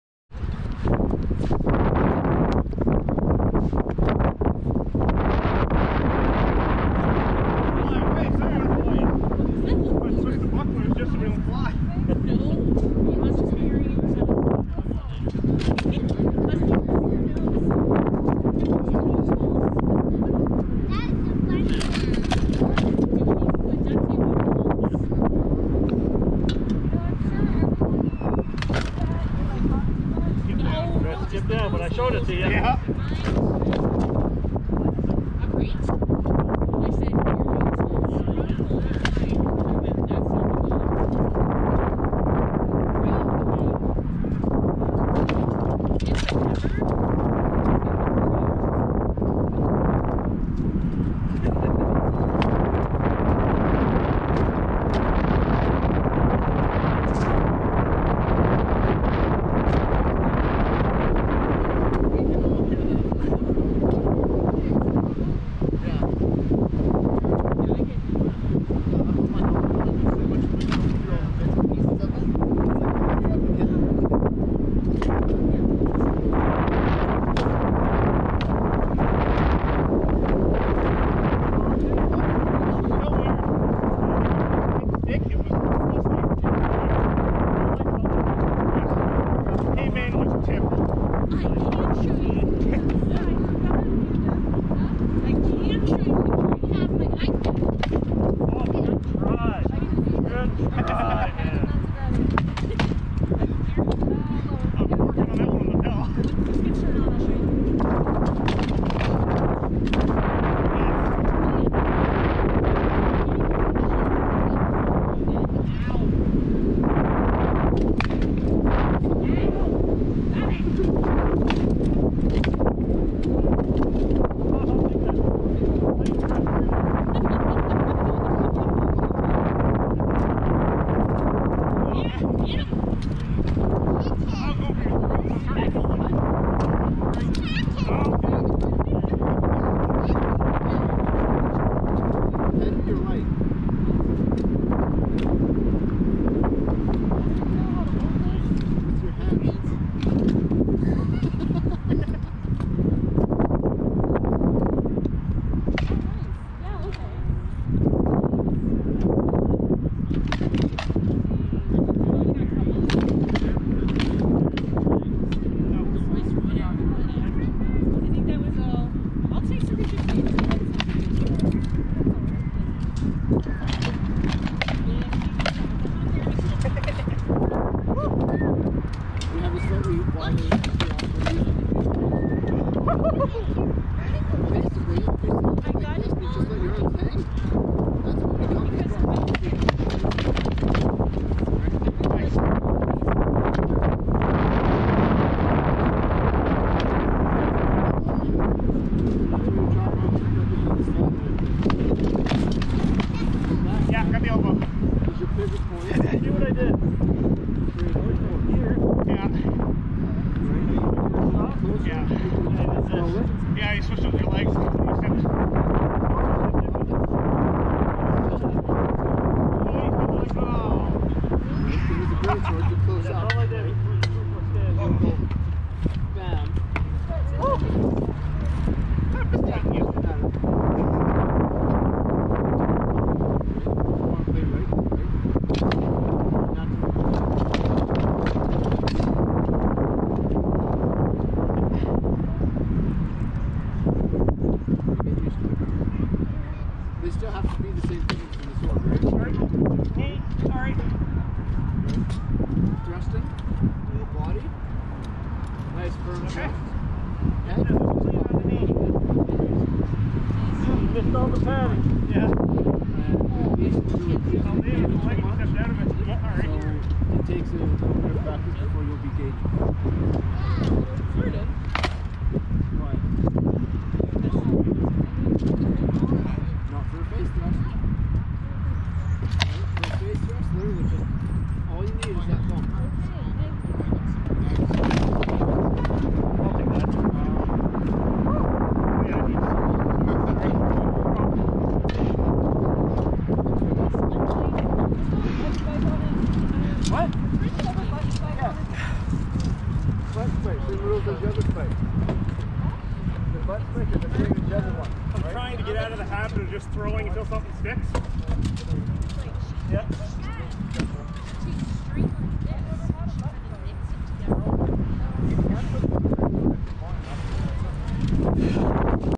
04-12 Sir Spurius vs HL Thorin
With the world slowly starting to open, we are having small practices within local limits.
Please forgive the wind noise.
Color corrected from previous.
The rest of the footage tonight was outside the view of the gopro.